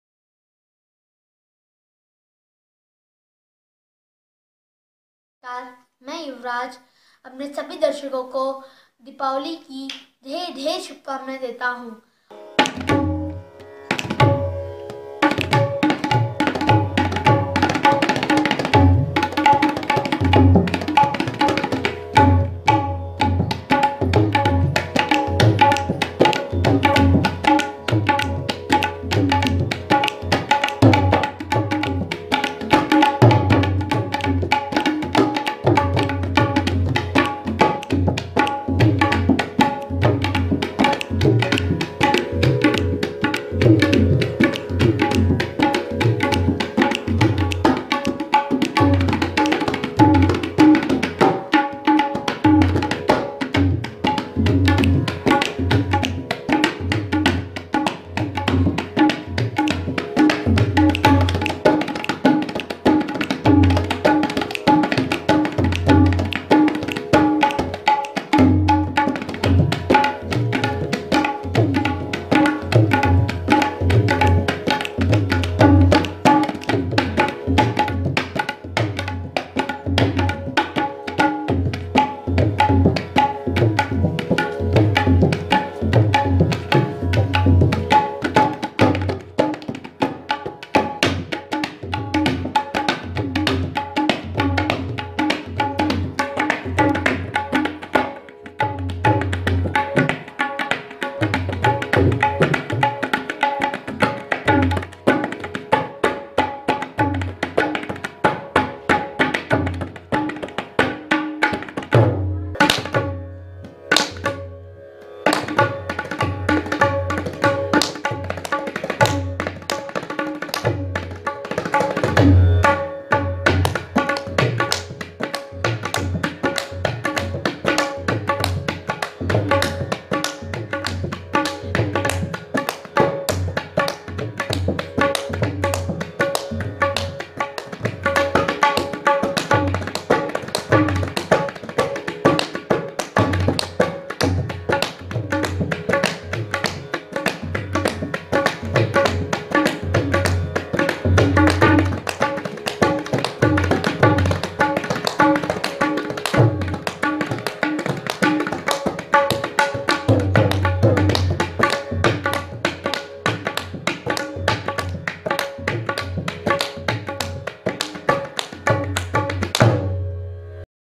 Tabla Cracker Rhythm | in Tal kaherwa by Yuvraj
Yuvraj wishing you very very happy and prosperous Diwali 2018 in tabla Rhythm you feel the sound of cracker if you listen it complete with headphone.